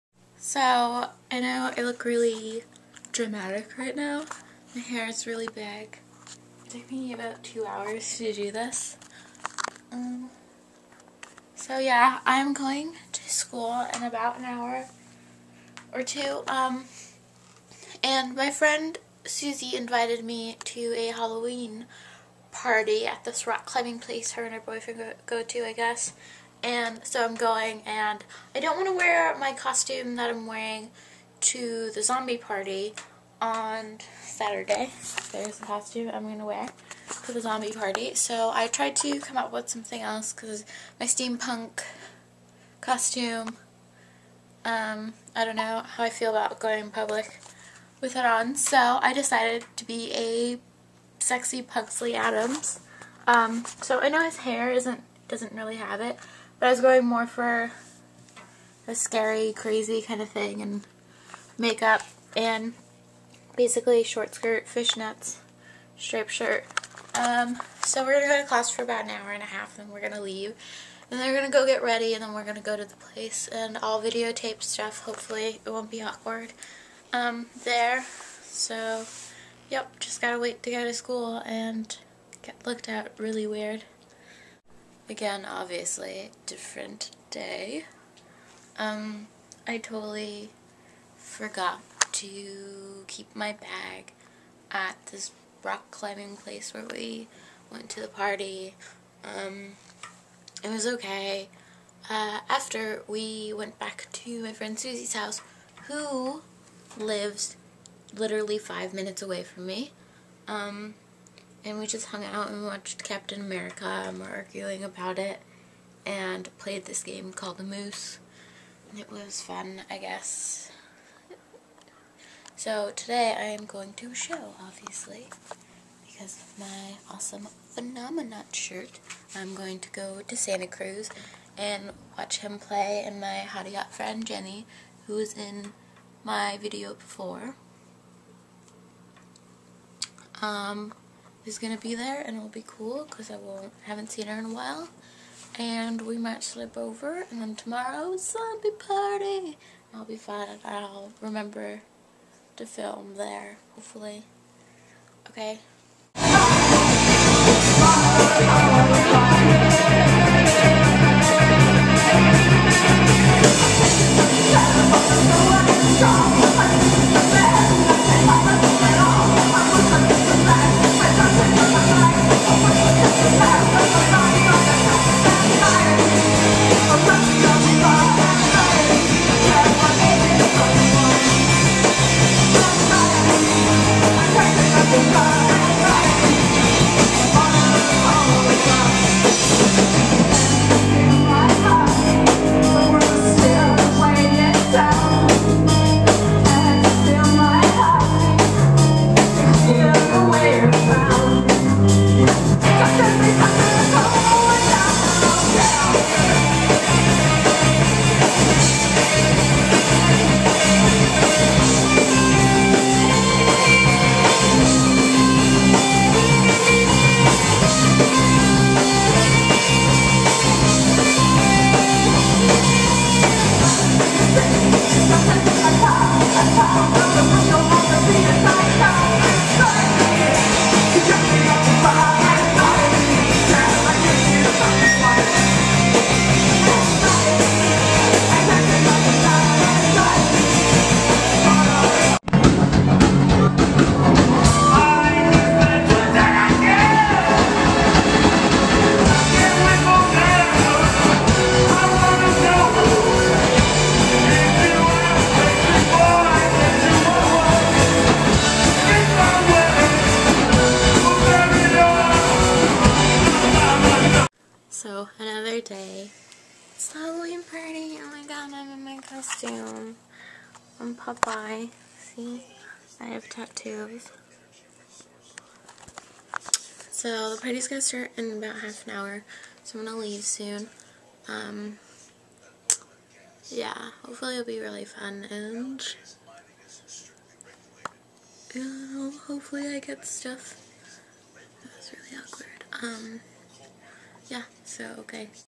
GOSH DARN KEYBOARD
MY KEYBOARD IS BROKEN AND ONLY WORKS WHEN THE CAPS LOCK IS ON. GOING TO GET IT FIXED. 3 DAYS IN ONE VLOG. I AM SO LAZY.